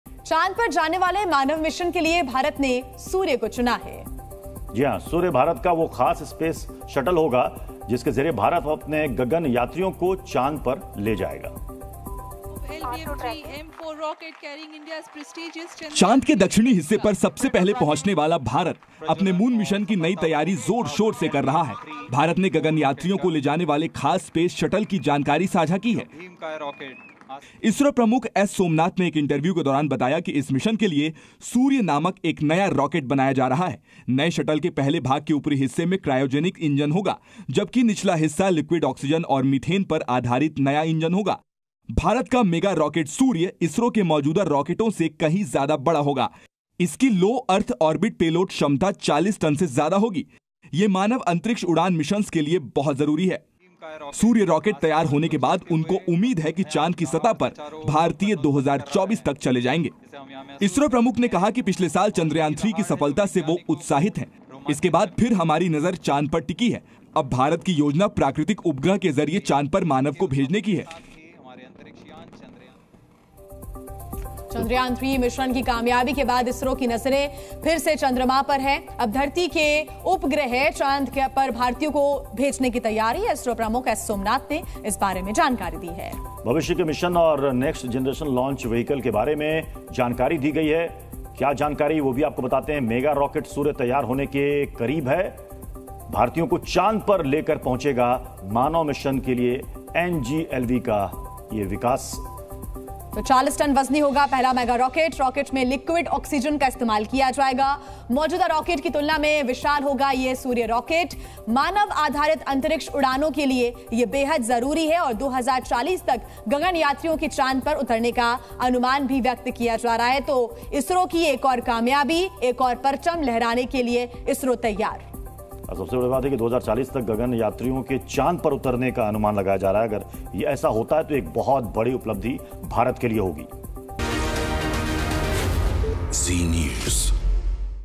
Moon Mission Update: चांद पर लेकर जाएगा 'सूर्य'? | Chandrayaan | SS Somnath | ISRO | Hindi news
Moon Mission Update: ISRO की मुहिम जारी है. 'सूर्य' रॉकेट तैयार होने के बाद उम्मीद है कि चांद की सतह पर भारतीय 2024 तक चले जाएंगे. चंद्रयान-3 की कामयाबी के बाद नजरें फिर से चांद पर हैं. धरती के उपग्रह चांद पर भारतीयों को भेजने की तैयारी है. इसरो चीफ ने फ्यूचर मिशन और नेक्स्ट जेन लॉन्च व्हीकल के बारे में जानकारी दी है.

Moon Mission Update: ISRO's campaign continues. After the 'Surya' rocket is ready, it is expected that Indians will go to the surface of the moon by 2024. After the success of Chandrayaan-3, eyes are again on the moon. There are preparations to send Earth's satellites to the Moon and Indians. ISRO Chief has given information about the future mission and next generation launch vehicle.